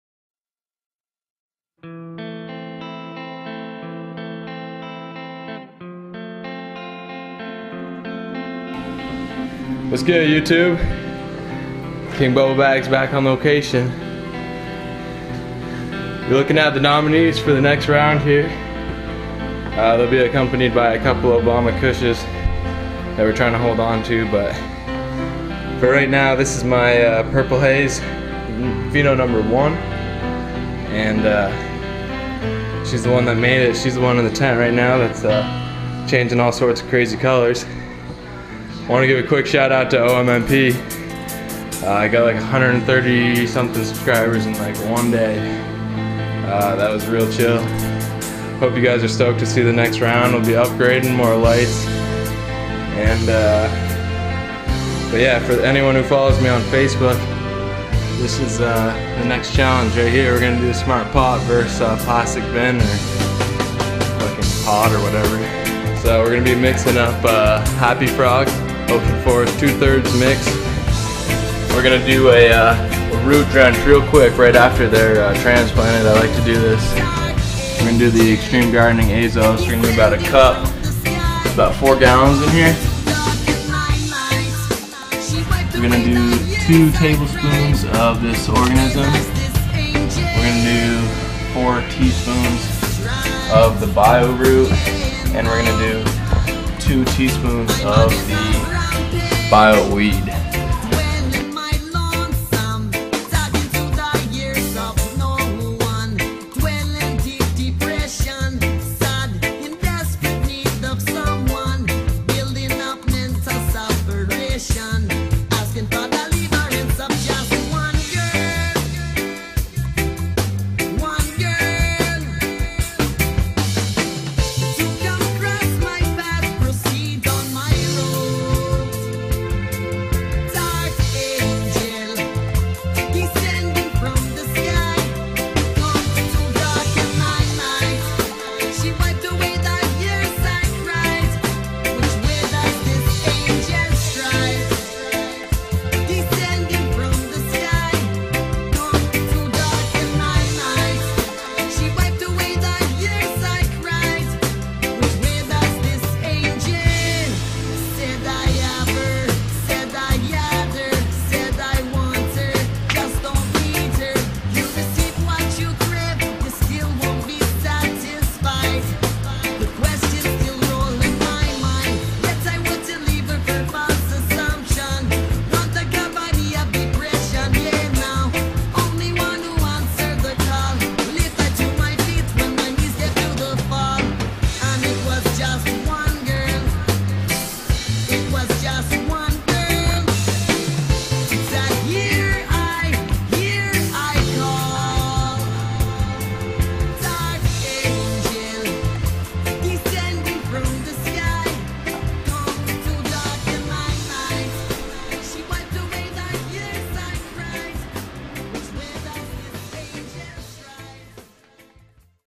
Episode VIII: Smart Pots vs. Plastic Pots
2/3 Happy Frog/Ocean Forest

Transplant Root Drench - 4 gal
1 cup - azos
2 tablespoons - organism xl
4 teaspoons - GO bio roots
2 teaspoons - GO - bio weed